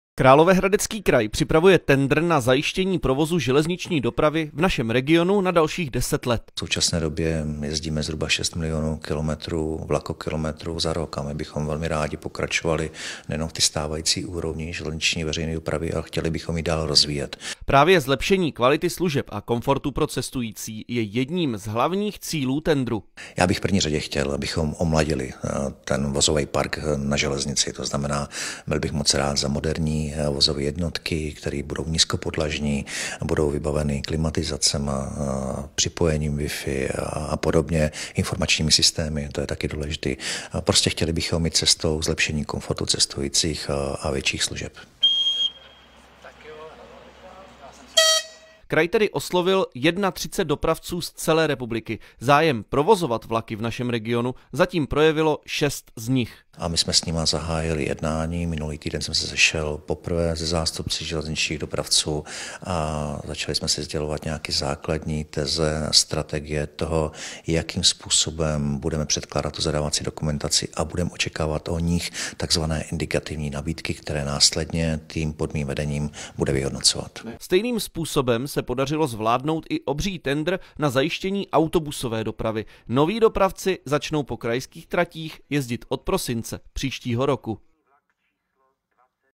Královéhradecký kraj připravuje tendr na železniční dopravce
„Chtěli bychom jít cestou zlepšení komfortu cestujících a služeb ve vlacích," říká k připravovanému drážnímu tendru první náměstek Martin Červíček. Po úspěšném autobusovém tendru kraj nyní hledá drážní dopravce, kteří by v našem regiony zajišťovali veřejnou vlakovou dopravu pro dalších 10 let.